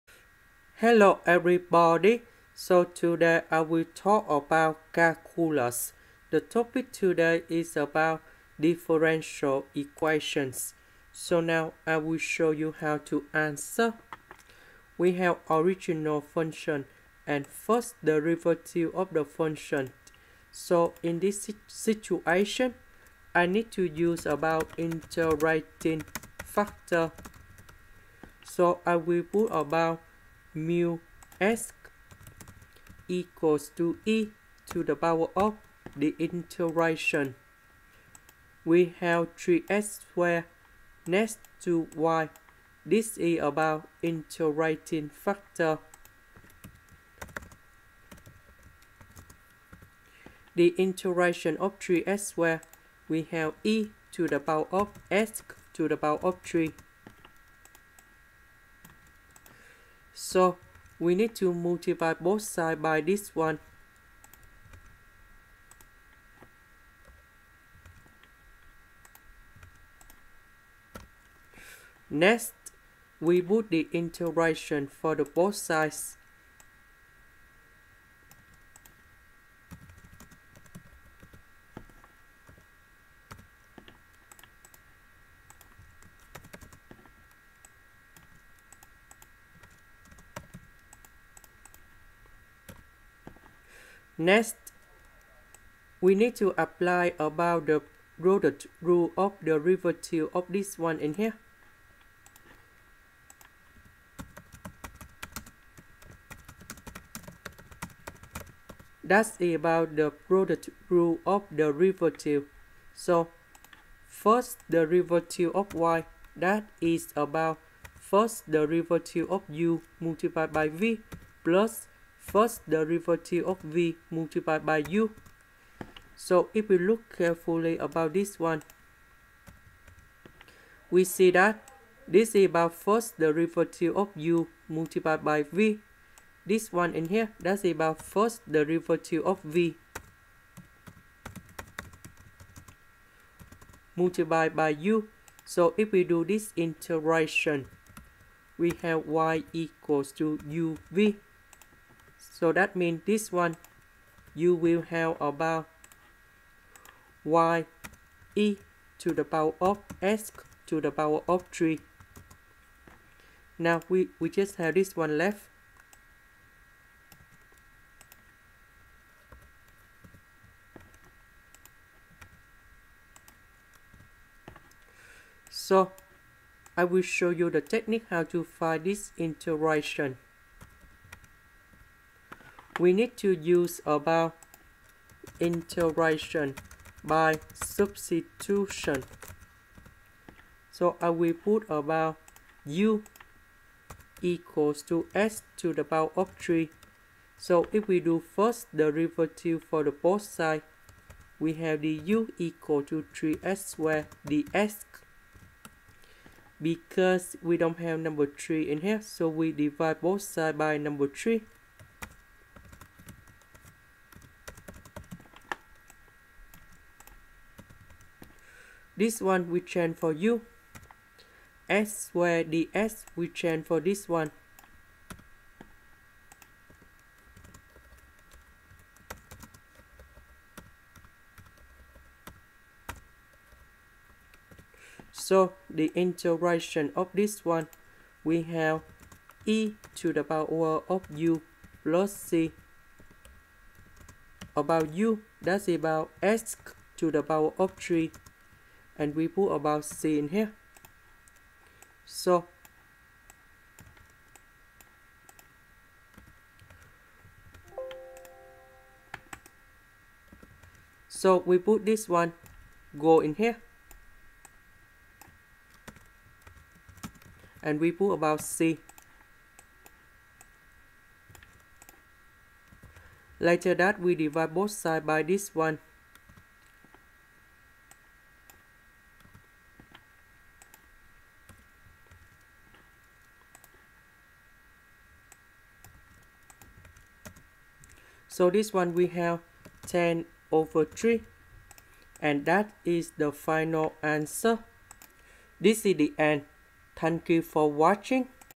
Calculus Help: Integrating Factor - Differential Equation - y'+3x^2 y=10x^2 - Techniques
Here is the technique to solve this differential equation and how to find them in here